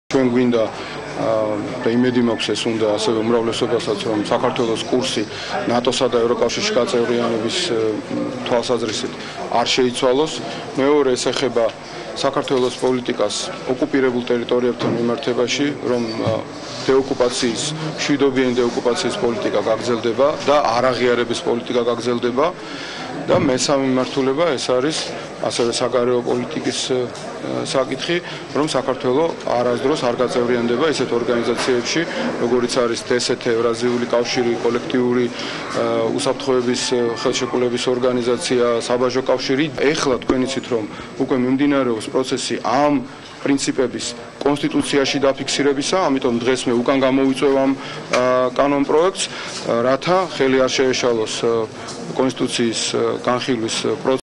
გიორგი ბარამიძე
საპარლამენტო უმცირესობის წევრი გიორგი ბარამიძე საგარეო კურსის უცვლელობასთან დაკავშირებულ კანონპროექტზე საუბრობს